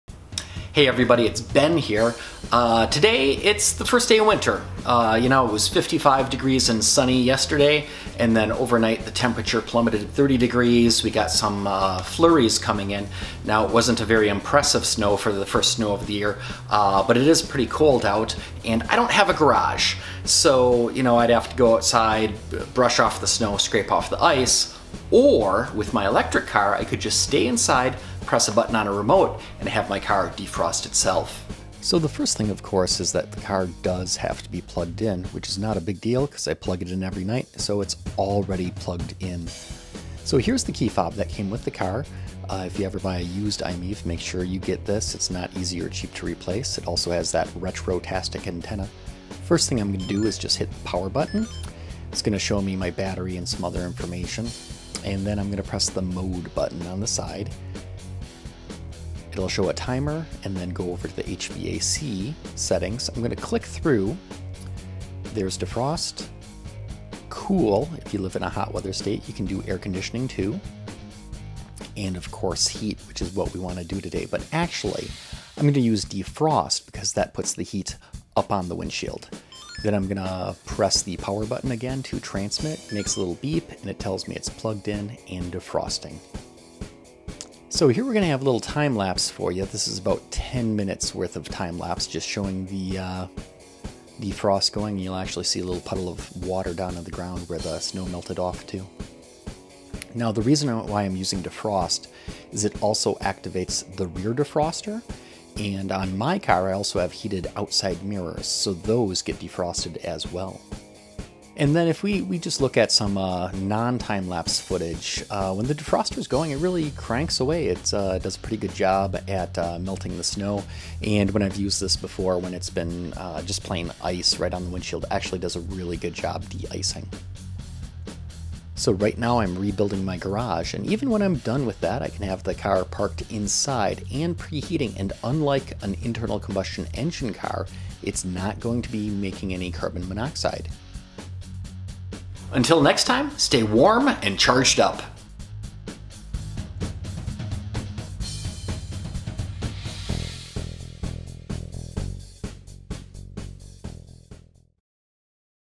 Electric Car Remote Defrost Feature Demo
I show how the winter Remote Defrost feature works on my Mitsubishi iMiEV electric car. Nearly all Plug-In cars have some sort of similar feature, allowing you to hop into a nice warm car all winter long. (Or start the air conditioner in the summer in hot weather!) Many cars use the touch-screen control in the car, a smart phone app, or a timer. In this case, I'm just showing how this works with a fob that came with the car.

If you are considering purchasing a used iMiEV, make sure this fob is included! On this car, it's the ONLY way to access the remote heat feature! Replacing it at the dealership is troublesome and expensive!

PS: Apologies for the autofocus problem at the beginning of the video. This is a camera I haven't usually used. Also, when I show the close-up of the fob, the car's battery appears low. I shot this late in the day after I had done all my driving. Normally, I preheat on winter mornings, the car charged all night, and the battery is full. That way, I leave with a full battery and a warm car. All power for the pre-heat comes from the wall, not from the battery. This helps winter driving range and improves driver comfort.